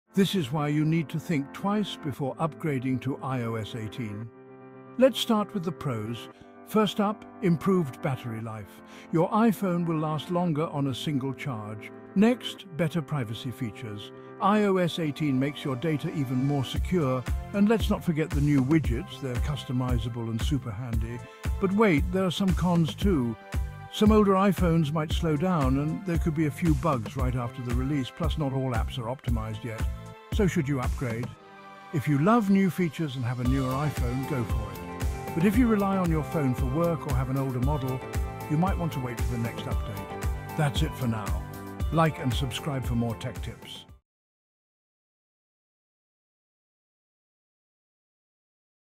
IOS 18 has some INSANE new features
Discover the latest features in our new software update! 🆕 In this video, we dive into three major enhancements that will elevate your user experience.

1. Improved Battery Life 🔋: Say goodbye to frequent charging with our optimized battery performance.
2. Better Privacy Features 🔒: Protect your data with enhanced security measures, making your device more secure than ever.
3. New Customizable Widgets 📱: Personalize your home screen with updated, interactive widgets that make accessing your favorite apps smoother and quicker.

OUTLINE:

00:00:00 iOS 18: Pros and Cons Explained!